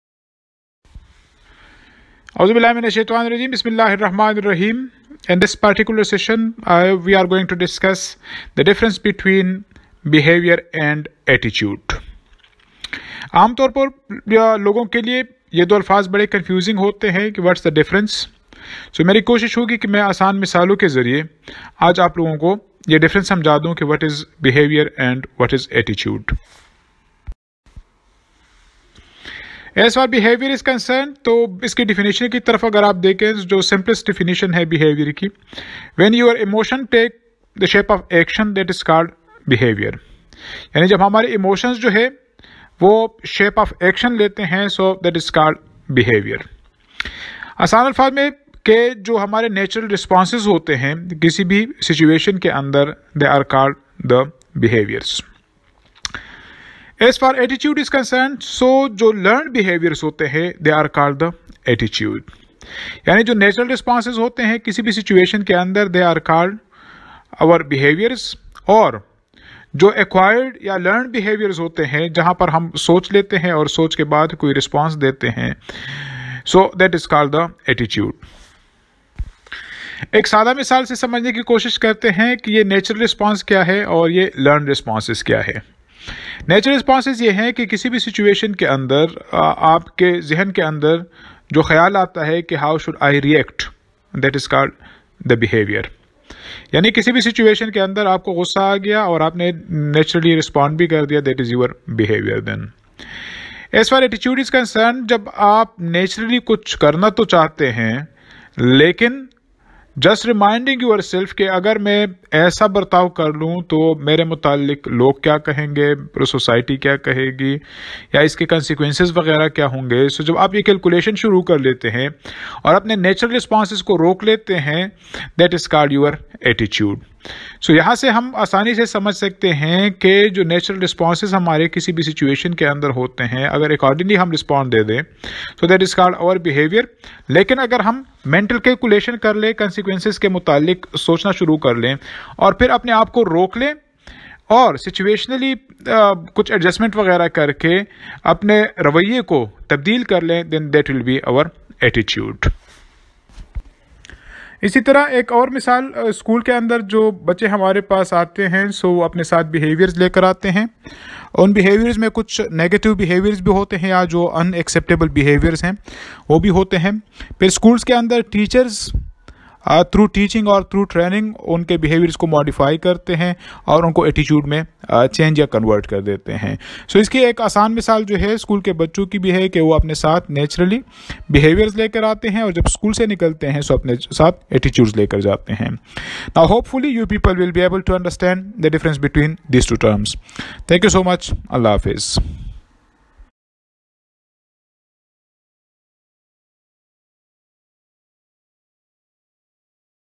Difference Between Behavior and Attitude
These two terms are often confusing. This short video will help in understanding the difference